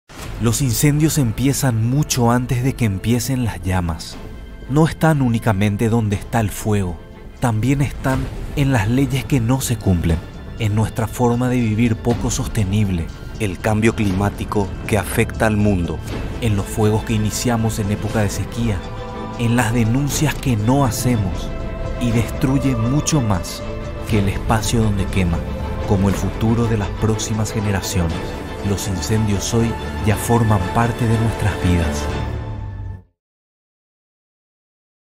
Campaña "Los Incendios hoy ya forman parte de nuestras vidas"
Campaña de prevención de incendios liderada por WWF-Paraguay denominada “Los incendios hoy ya forman parte de nuestras vidas”

El impacto de los incendios forestales en nuestra región tiene consecuencias globales. Para disminuir el riesgo de que desastres como estos se repitan con más frecuencia, es fundamental la generación y aplicación de políticas públicas que disminuyan las tasas de deforestación, ejerzan mecanismos de control eficientes para detectar delitos ambientales, apunten a gestionar la resiliencia de las comunidades al cambio climático, impulsen el ordenamiento del territorio desde los gobiernos locales, y promuevan buenas prácticas de ganadería y agricultura sostenible, estableciendo férrea protección a las áreas protegidas, y fomentando el trabajo conjunto desde la mirada transnacional hacia el cuidado de las ecorregiones.